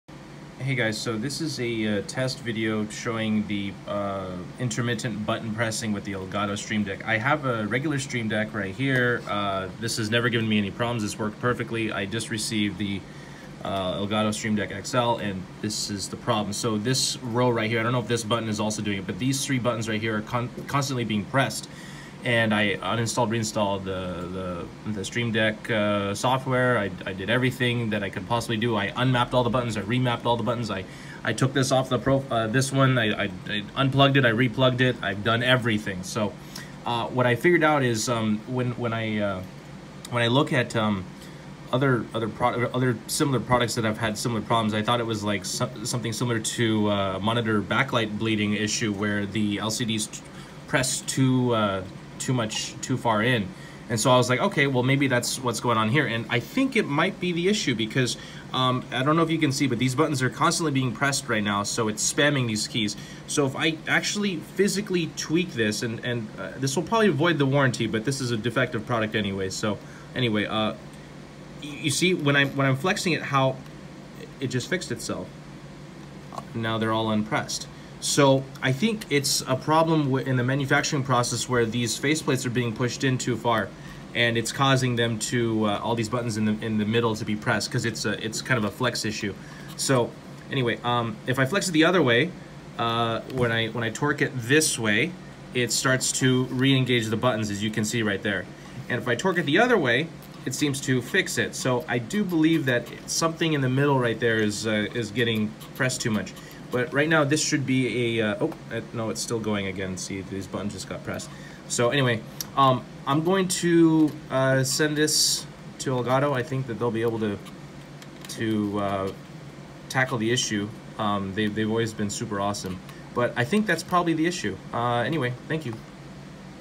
How to fix Elgato Stream Deck keeps pressing button problem
My streamdeck arrived and wouldn’t work properly. This is the fix that worked for me!